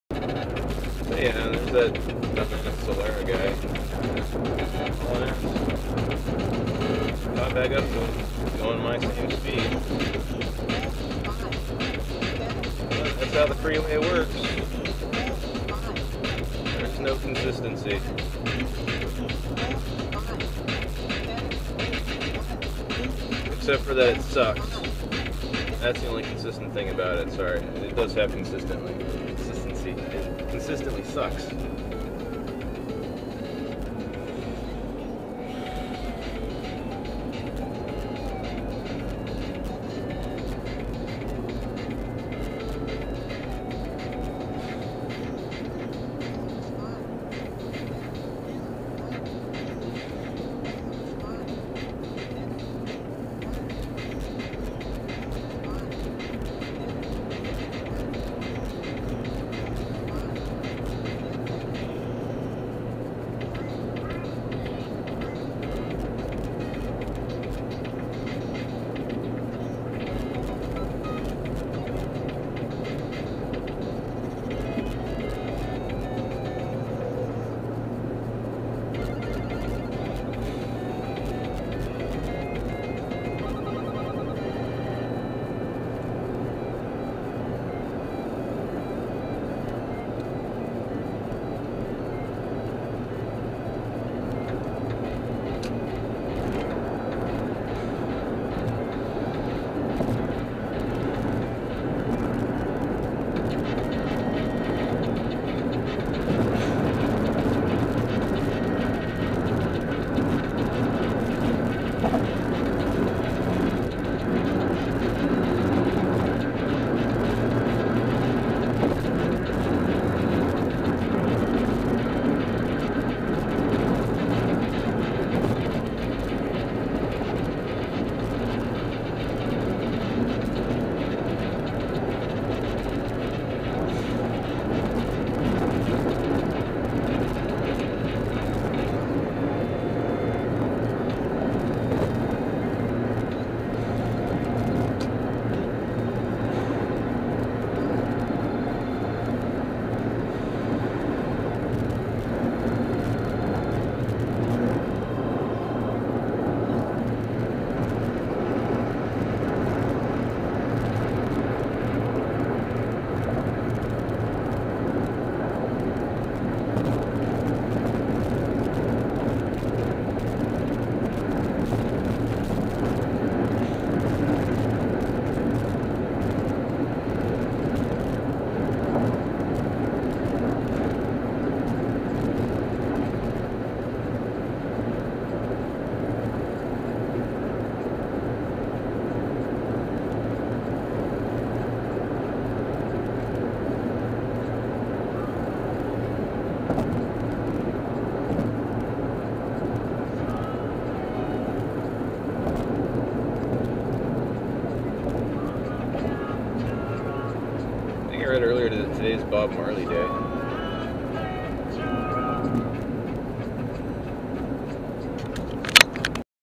2012.02.06 Daily Driving: Expensive Errand Medical Cannabis [9/11]
Did my expensive errand today. Ranted quite a bit. Forgot to go to the bank down in Laguna Hills, so I had to go to the other one in Tustin. Hurting considerably more than usual today, and the past few days. Driving around now with MORE commentary! Sometimes even special Guests. Just an example video from my Contour HD camera to show off the video quality to friends. You might want to turn down your speakers, the microphone is lacking.